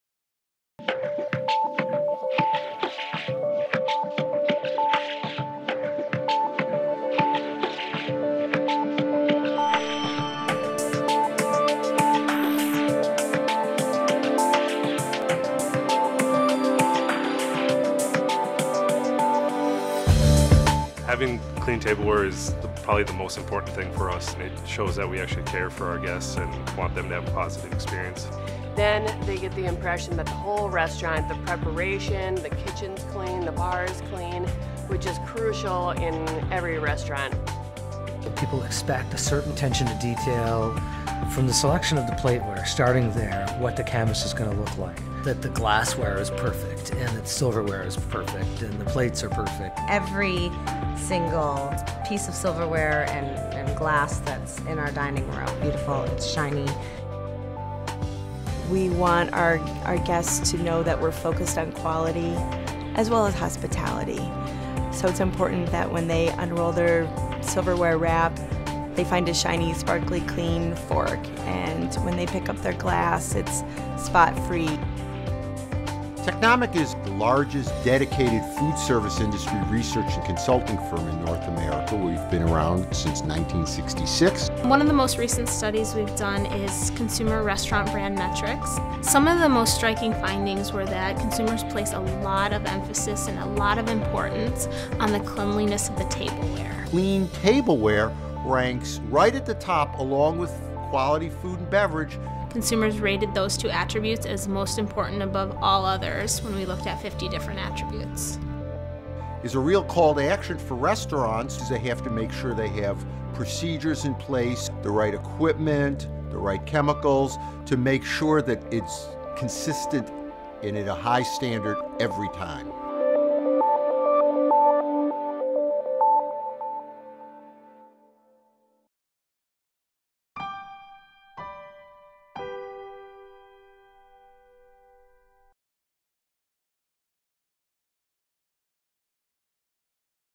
Why Clean Matters - Overview
Surprising new research from Technomic, a restaurant industry research and consulting firm, confirms what leading chefs and restaurateurs know intuitively—clean tableware is the number-one factor affecting restaurant guests' dining experiences. See why sparkling clean wine glasses and spotless silverware are key to ensuring diners walk away with positive impressions of restaurants—and keeping coming back for future visits.